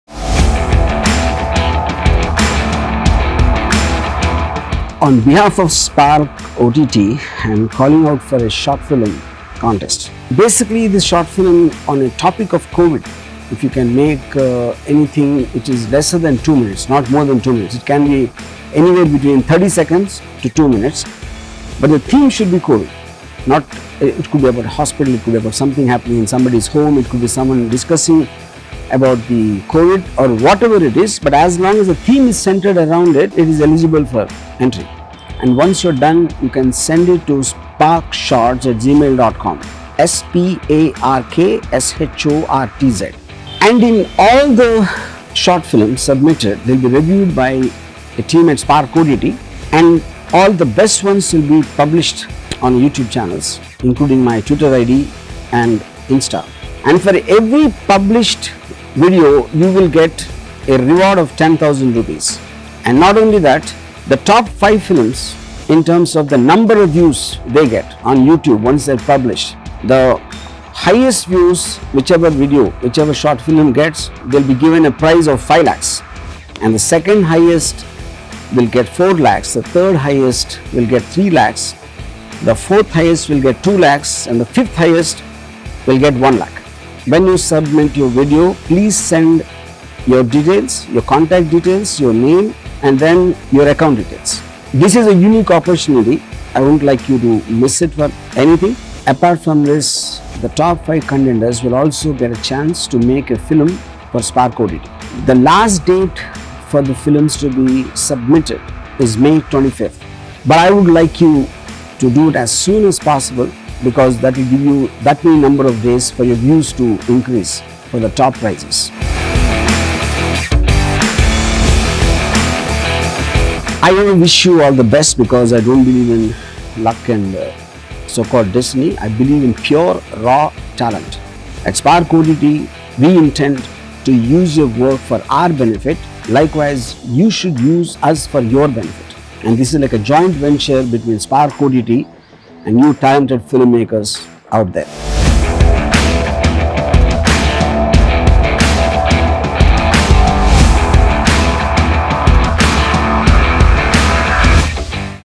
SPARK SHORT FILM CONTEST || RGV|| SPARKSAGAR || SPARKOTT STREAMING FROM MAY15TH
On behalf of Spark OTT ,RGV invites budding filmmakers to participate in a  short-film contest. You can make a short film of 2 mins or less on the theme of Corona and its related stories.
Every selected entry will be uploaded in RGV and Spark Social media handles and will be awarded Rs 10,000/- .

The top 5 entries in number of views garnered will win: Rs. 500,000 for highest views, Rs 400,000 for second highest views  Rs. 300,000 Rs 200,000 for 4 th highest  and 100,000 for 5th  Number of views time line will be based upon the highest views garnered in the first 10 days of upload.
For example if your entry is selected and uploaded on 15 th May 12 noon ,The final views for the contest will be counted on 25 th May 12 noon. Also along with the cash prizes the top 5 winners will be given a chance to make a film for Spark OTT

Last date to send in your entries is May 25th. Results will be published on June 10 th along with the details of names and photos